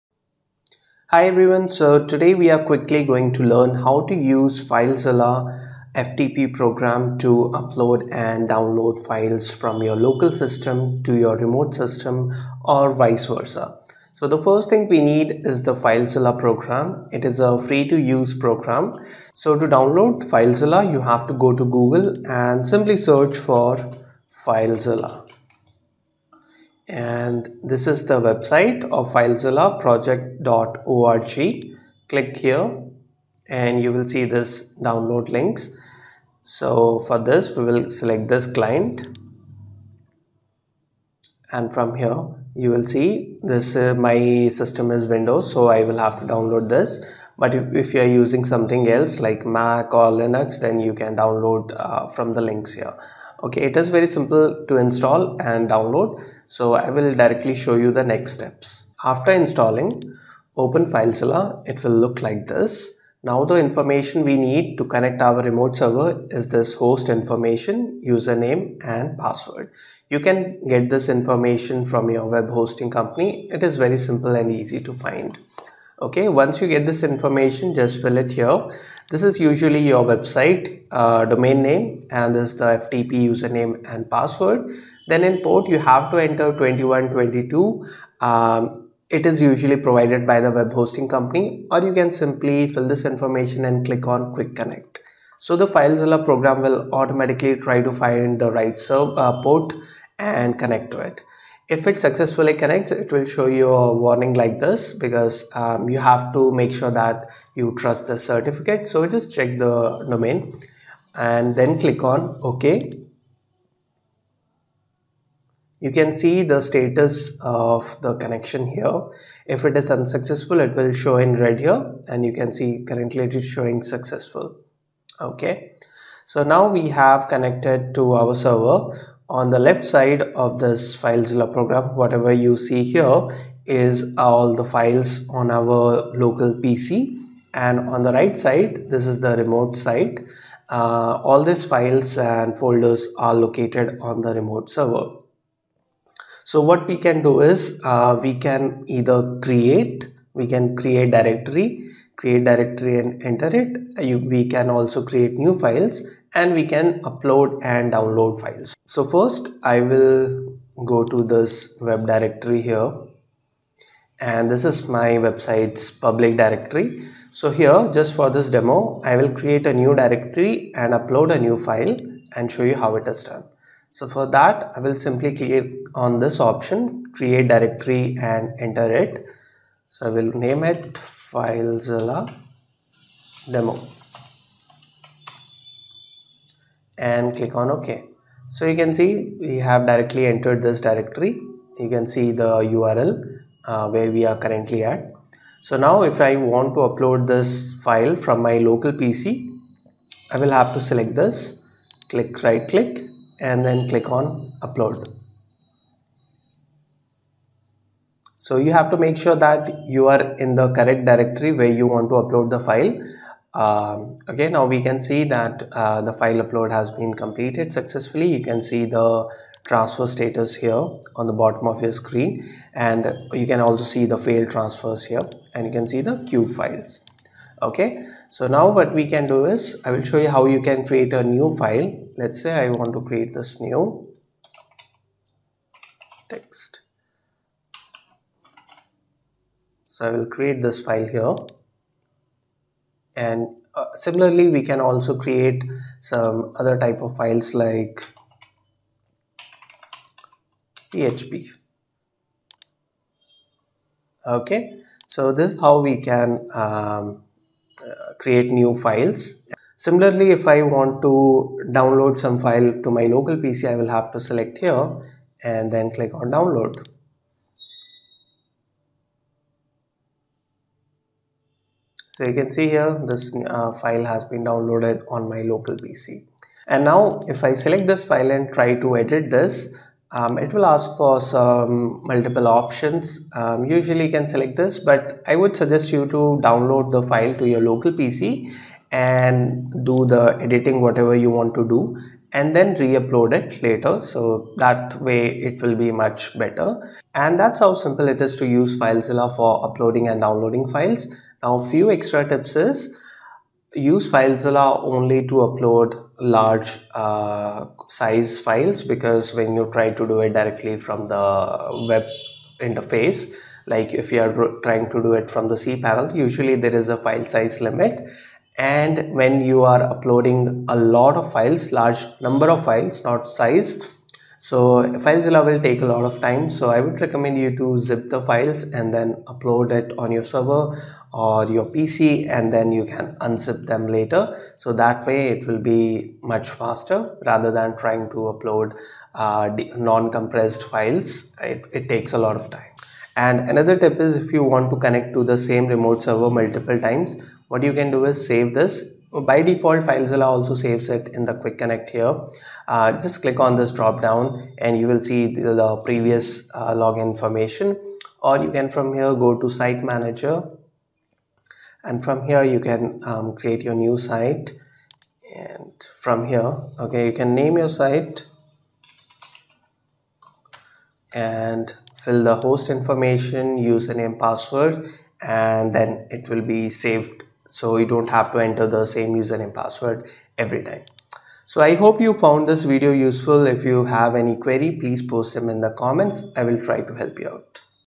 2024 - How to use Filezilla FTP to Upload Transfer Files to any Website Server
In this video you can learn how to use filezilla free FTP program to upload, download and transfer files between your local system and remote website web server. This method will work for any vps, cpanel or other web server management system.

Please post any queries in comment section.